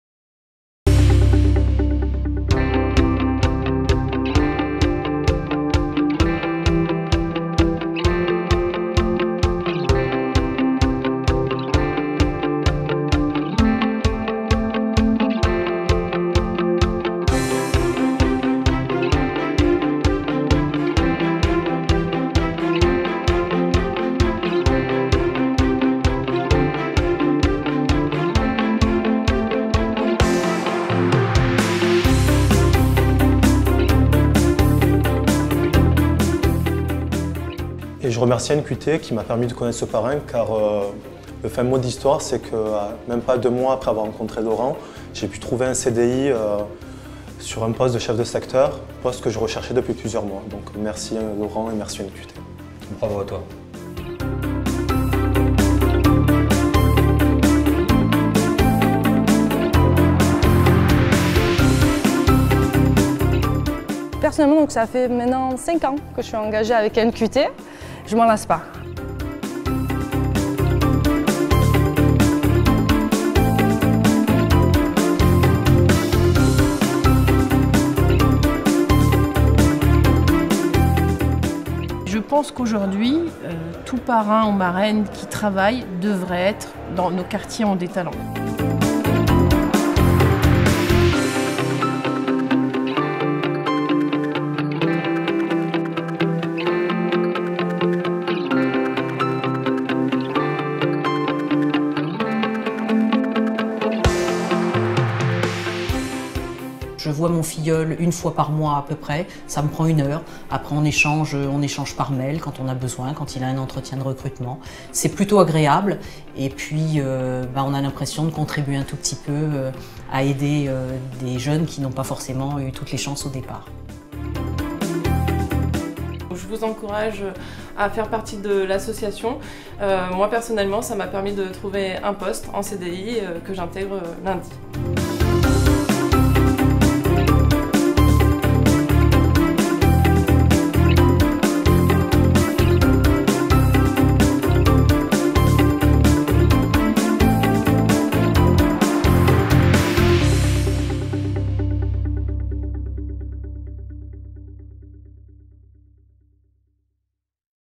Nos Quartiers ont des Talents - NQT
Vidéo de présentation de l'Association NQT "Nos Quartiers ont des Talents" réalisée par Abaques Prod.

L'association NQT accompagne individuellement et efficacement les jeunes diplômés vers l'emploi, en s'intégrant à la politique sociétale de ses entreprises mécènes et à l'engagement de ses partenaires publics.